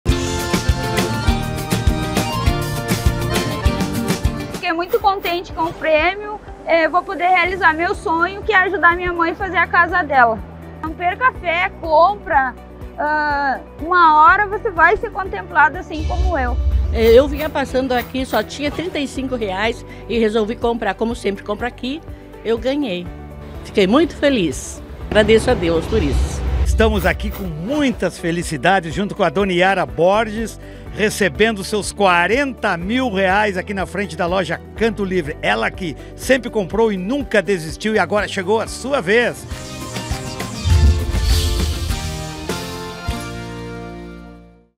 Ganhadores Fiat Mobi + 20 Mil Reais - Trilegal Tchê - 28/08
Teve muita gente feliz da vida! Confira os ganhadores que dividiram um Fiat Mobi + 20 Mil Reais no sorteio do dia 28/08.

Parabéns aos felizardos!

Confira onde cada uma garantiu seu Trilegal Tchê.

Título de Capitalização somente para o interior do Rio Grande do Sul - Brasil.